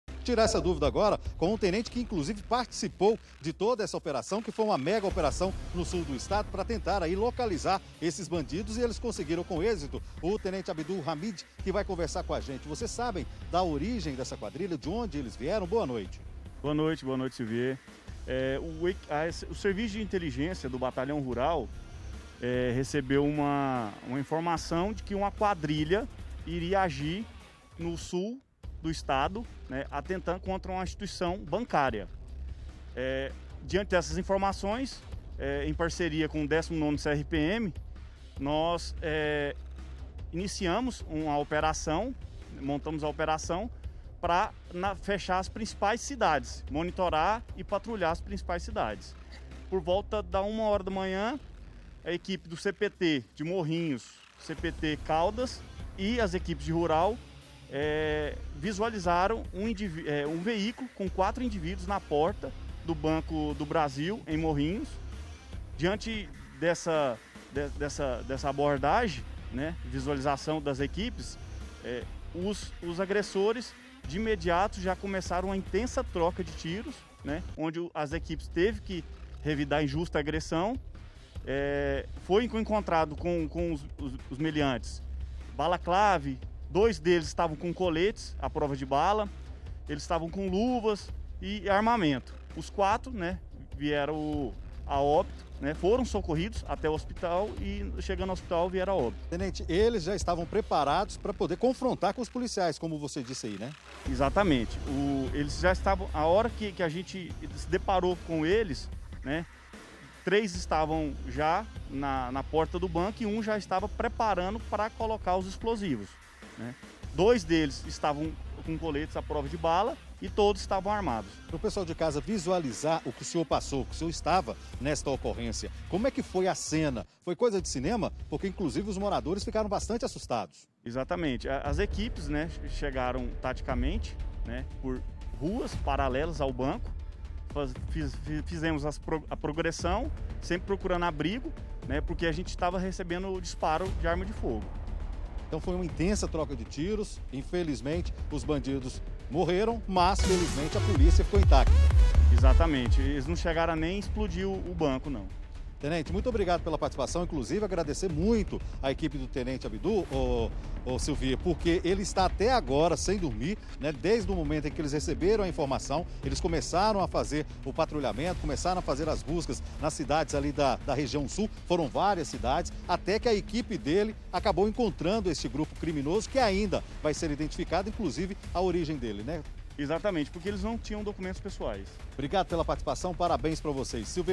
TENSÃO! LADRÕES EXPLODEM BANCO E MORREM EM CONFRONTO
BANDIDOS EXPLODEM BANCO E TROCAM TIROS COM A POLÍCIA. QUATRO MORRERAM.